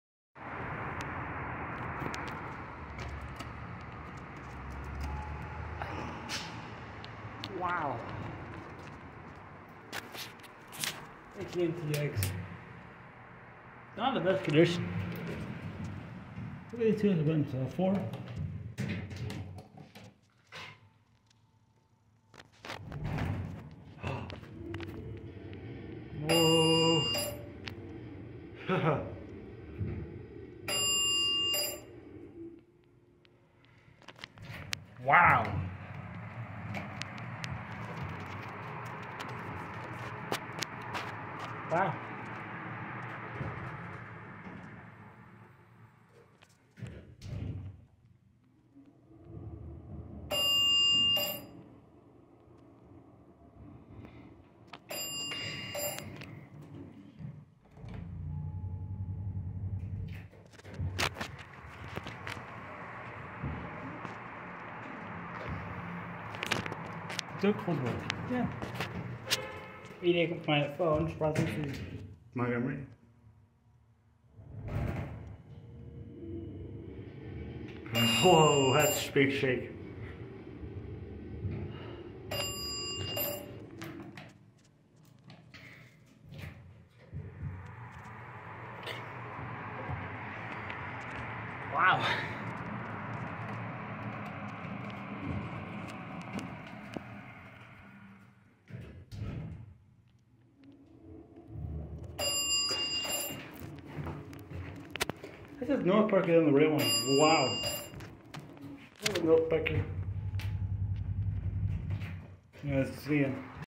BIG SHAKE! | used Montgomery | Dillard's garage, Northpark mall, Dallas Texas
apparently had a big jolt going up for a while
RUNTIME: 1:54